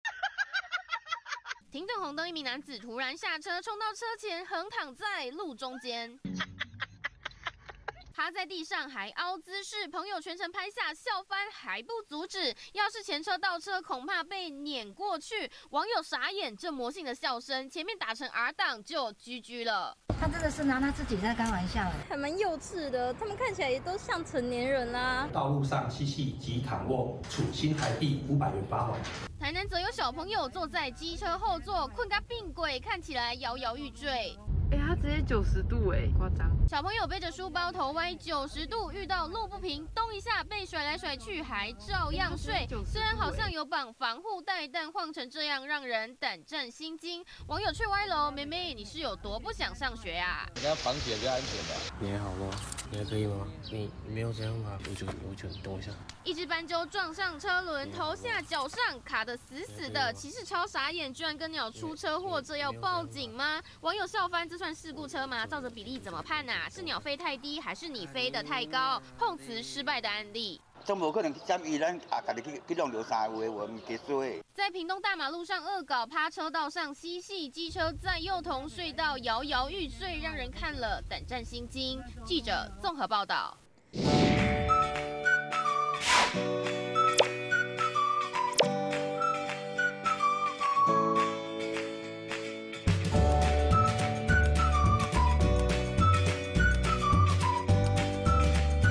【每日必看】後座童睡翻90度歪整路 搖搖欲墜直擊驚呆｜趴地險輾友魔性笑不阻止 橫躺路中凹姿勢自拍 20240624
屏東內埔出現馬路亂象，一名男子趁紅燈，下車衝到馬路中間，趴在車道上還凹姿勢，車上友人狂笑還拍下影片，要是前車倒車恐怕被輾過去，台南有媽媽騎車載小孩，後座小朋友卻整個睡翻，頭歪90度搖搖欲墜，看起來超級危險。